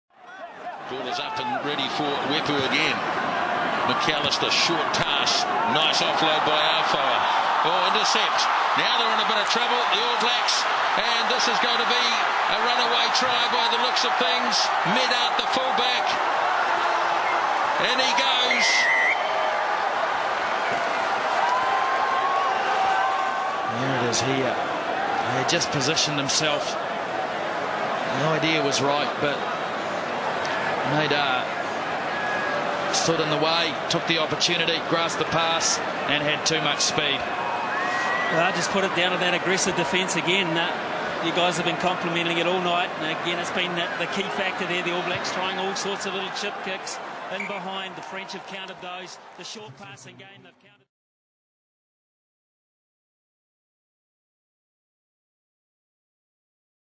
Le jour où Maxime Médard crucifia les Blacks sur leurs terres. (2009)
Nouvelle-Zélande - France, Test match, Juin 2009.